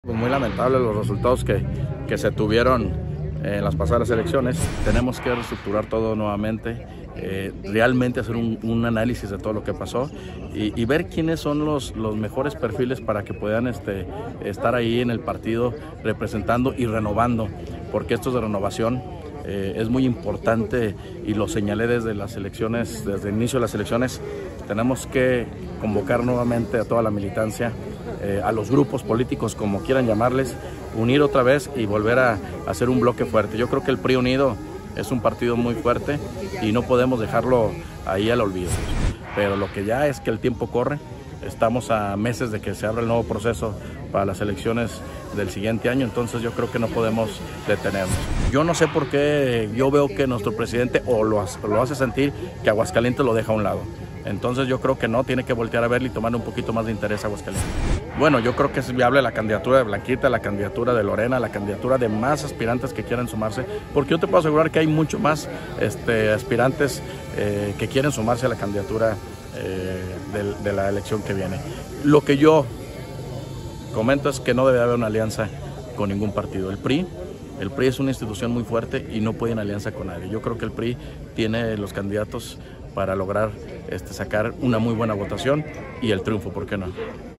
PRI no deben convertirse en un partido satélite o comparsa
Luego de calificar como francamente lamentables los resultados obtenidos por el Partido Revolucionario Institucional (PRI), durante el pasado proceso electoral, reconoció la urgente necesidad de entrar a un proceso de renovación y reestructuración que le permita recuperar algo del terreno perdido y no convertirse en simple comparsa o satélite de otros organismos partidistas.
El excoordinador de la breve campaña de Blanca Rivera Rio de Lozano a la Presidencia Municipal capitalina hizo un llamado a centrarse en escoger los mejores perfiles para los comicios del próximo año, que deberán ser contra reloj y a pesar que la dirigencia nacional de su partido se ha olvidado del tricolor en Aguascalientes.
Finalmente, consideró que su partido no deberá realizar alianza alguna con otros partidos políticos durante el proceso electoral del próximo año.